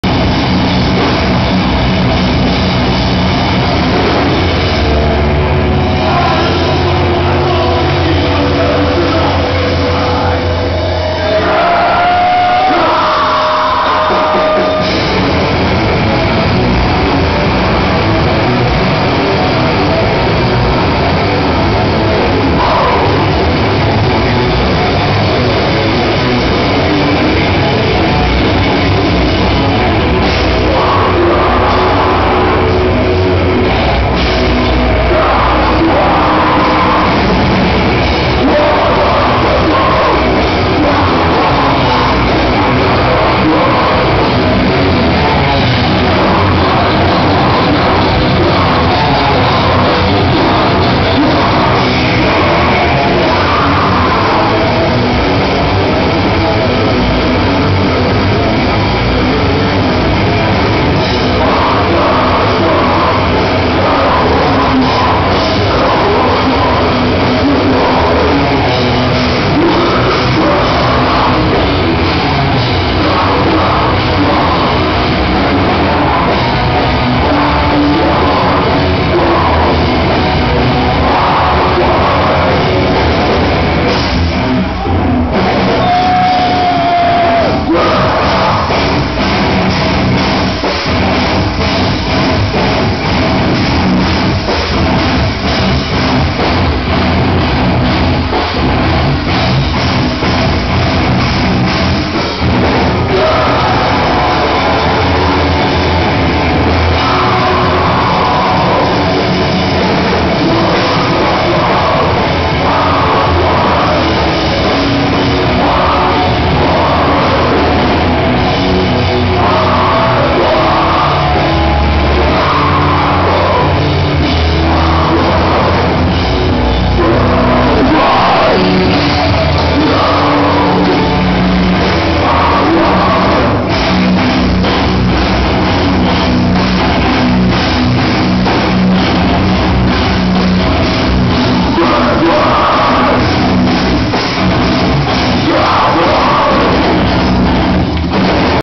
ARISE AND RUIN/Westville, Nova Scotia/Oct.12th 2007
The boys rip it up with the last song of the night. Sorry about the poor lighting and moving around.